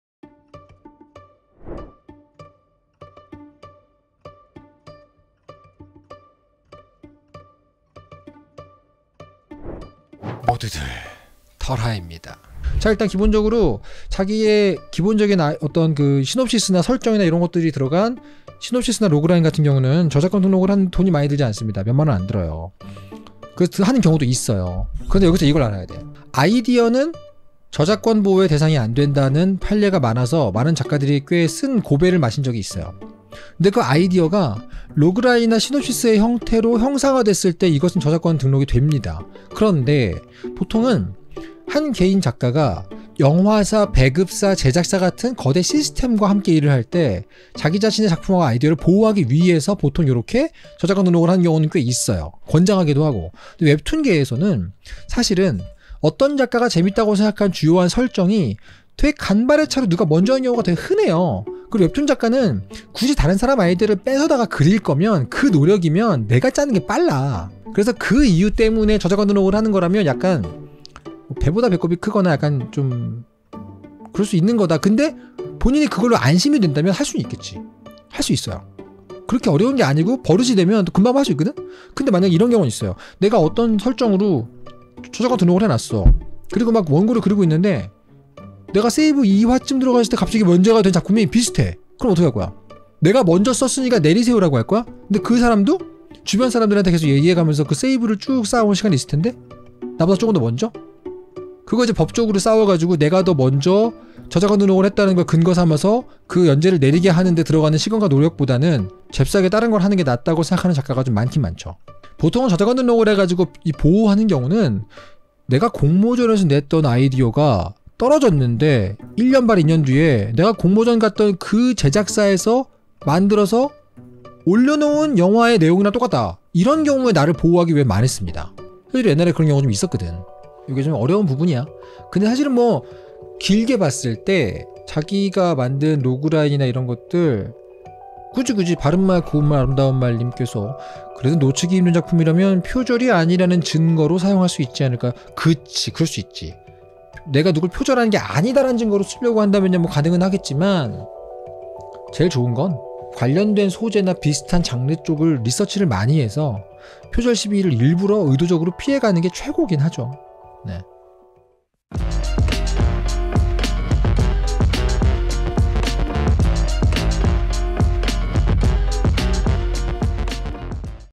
구상한 스토리중 괜찮은거는 저작권 등록을 해야 할까요?
본 영상은 4월 12일에 진행된 라이브 영상 편집본입니다.